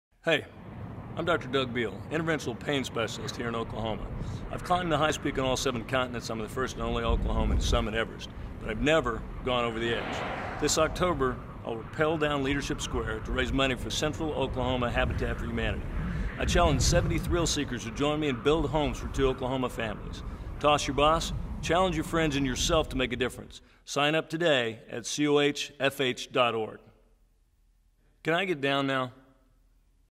Over the Edge Central Oklahoma Habitat for Humanity
Up to 76 Oklahomans can participate in a once in a lifetime opportunity to go Over the Edge for Central Oklahoma Habitat for Humanity, rappelling 16 stories down Leadership Square in downtown Oklahoma City on Saturday, October 7, 2017.

“As a non-profit, we always look for new ways to raise additional revenue to help more families. We jumped on the opportunity to bring Over the Edge to Oklahoma City because it allows supporters to take part in a thrilling experience,” said Ann Felton Gilliland, CEO & Chairman of Central Oklahoma Habitat. “Our goal is to raise $180,000, which pays for home builds for two Oklahoma families.”

Individuals must raise a minimum of $2,000 for the cause to secure their rappelling position. To register, sponsor, or donate, visit Central Oklahoma Habitat’s click on Over the Edge, and create a personal fundraising page, which allows participants’ friends, family, and colleagues to "sponsor" a rappel.

Rappels will begin at 8:00 a.m. and continue until 4:30 p.m. All participants will be treated like VIPs while their supporters cheer them on during their descent. It will take each participant approximately 5-to-15 minutes to get the bottom of Leadership Square.

Event sponsors include 7-Eleven, Pearl’s Restaurant Group, Guernsey Engineers/Architects/Consultants, KOCO News Channel 5, and Tyler Media’s The Franchise, Jake FM and The Edge. Event spokesperson Dr. Doug Beall, Spine Surgeon at the Oklahoma Spine Hospital, has committed to sign up and is ready to go.

“It’s funny. I’ve climbed the highest peaks on seven continents and was even the first Oklahoman to summit Everest, but I’ve never gone Over The Edge of a skyscraper! This will be a

great experience – and especially because it helps two Oklahoma families realize the dream of home ownership, said Beall.

Central Oklahoma Habitat’s motto is: “Providing Oklahomans with a hand up, not a hand out.” Each qualified partner family must complete 300 hours of sweat equity, attend financial management classes, and learn about the responsibilities of home ownership by attending Homeowners College. Homes are then sold to the family for the cost of construction and financed with zero interest mortgages.